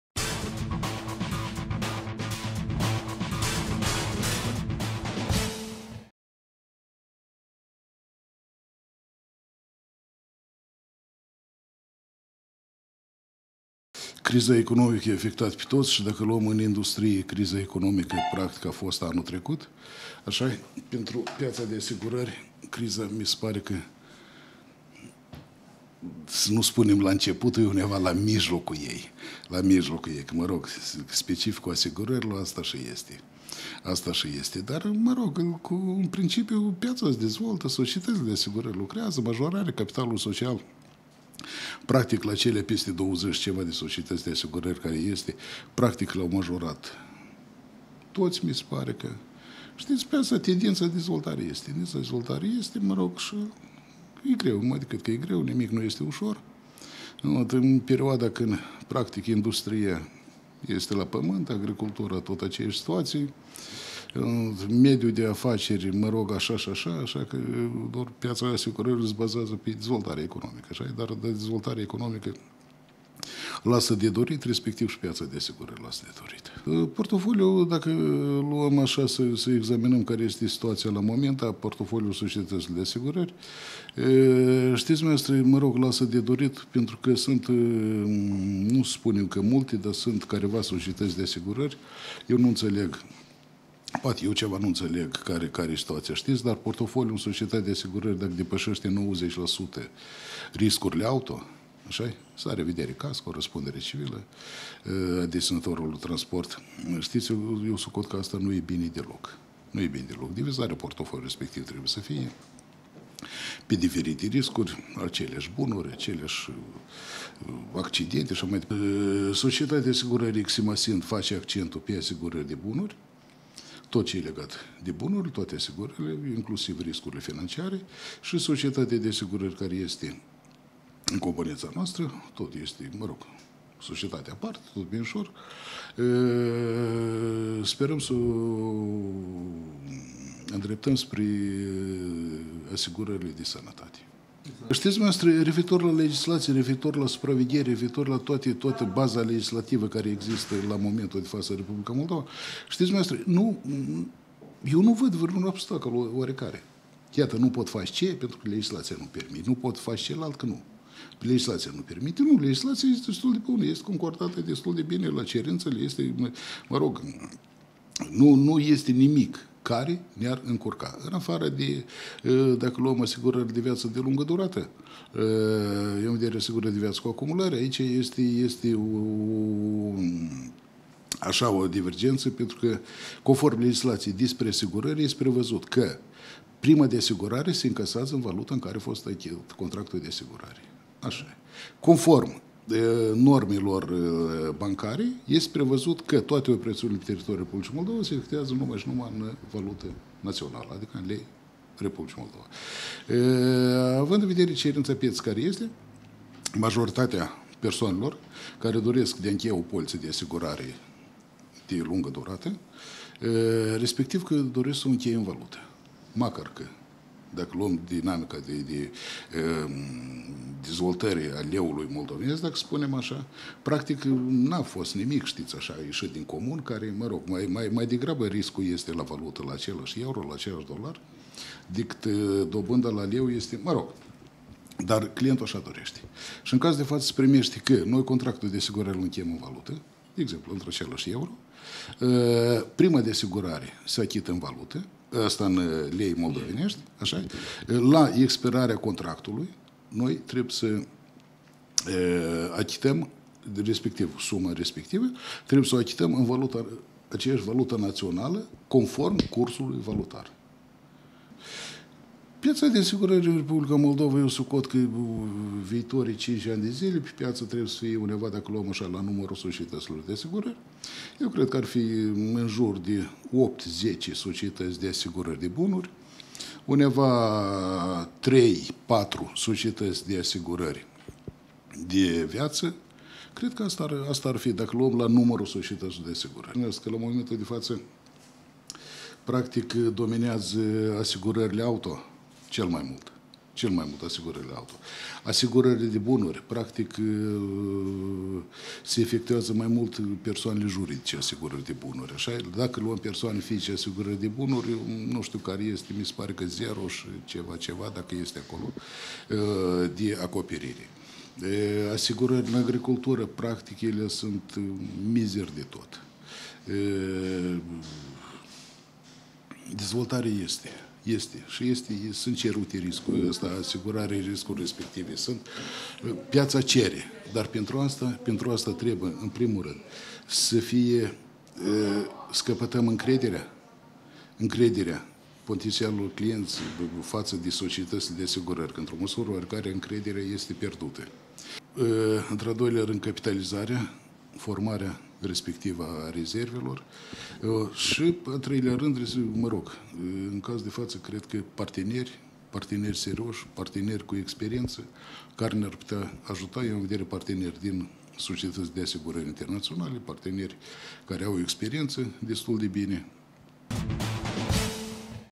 Mihai MAZARENCO, Director General, EXIM-ASINT
Chisinau, Republica Moldova
"Capitalizarea si formarea rezervelor este primordiala pentru asiguratorii moldoveni". Video original aici